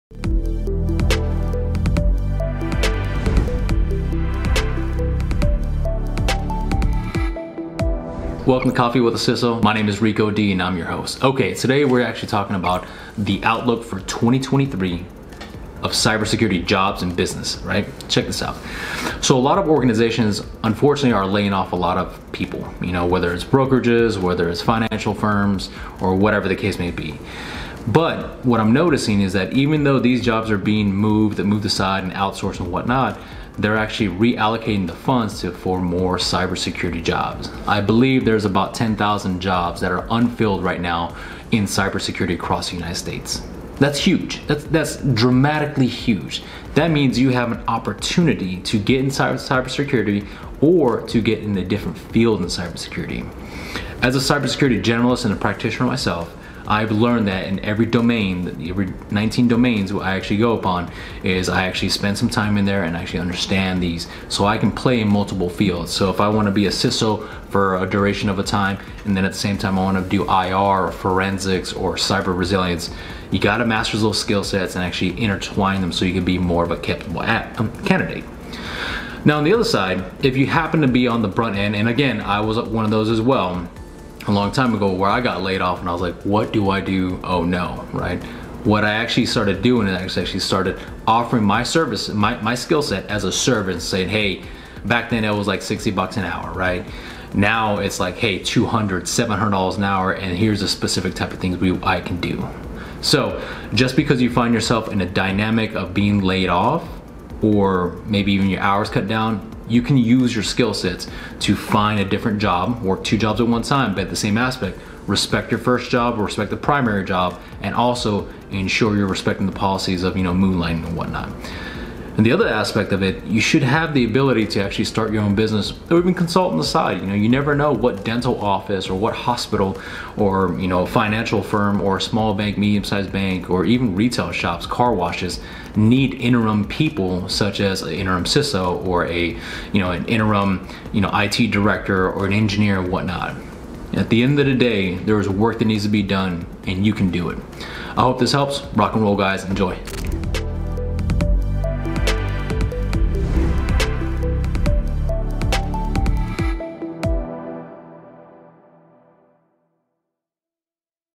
Cybersecurity Career Outlook 2023
In 2023 we are seeing layoffs in the Cybersecurity field. This is not good, and I do not wish this upon anybody. However, there is a silver lining here. There is an opportunity that you can offer your skill sets as a business. Who knows you might make a lot more money doing this. As businesses are laying of the labor workforce they are re-allocate the funds for services more specifically Cybersecurity.